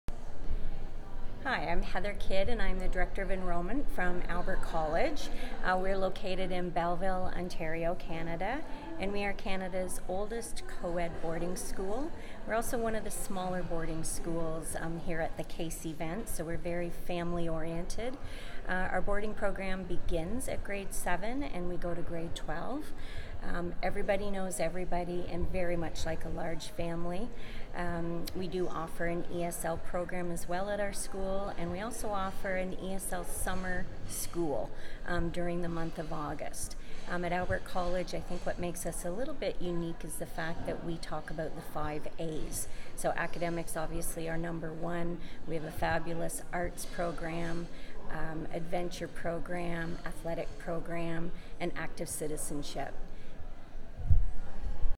CAIS Boarding Schools - Albert College
2019年11月18日 カナダ大使館でカナダ全寮制私立学校セミナーが行われ、参加校「Albert College」代表のHeather Kiddさんより、学校の紹介コメントをいただきました。